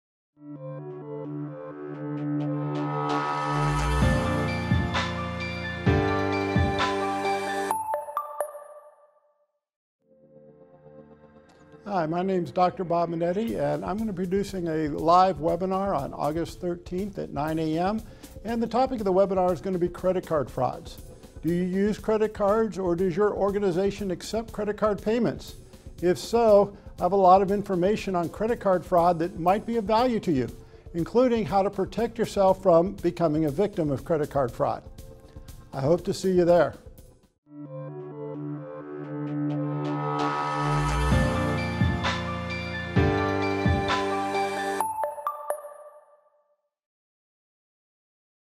Webinar Preview - Preventing Credit Card Frauds
This course is designed to review the issue of credit card fraud. Examples of actual frauds will be provided so participants can understand how the frauds are being committed and how they were uncovered. We will review various methodologies for detecting credit card frauds and will discuss developing internal controls to help prevent and detect credit card frauds. Examples will provided to show how criminals obtain and use credit card numbers. This course would be appropriate for CPAs, CMAs, CFEs, CIAs, CGMAs, MAFFs, staff accountants, accountants, managers, internal auditors, external auditors, bank employees and retail employees.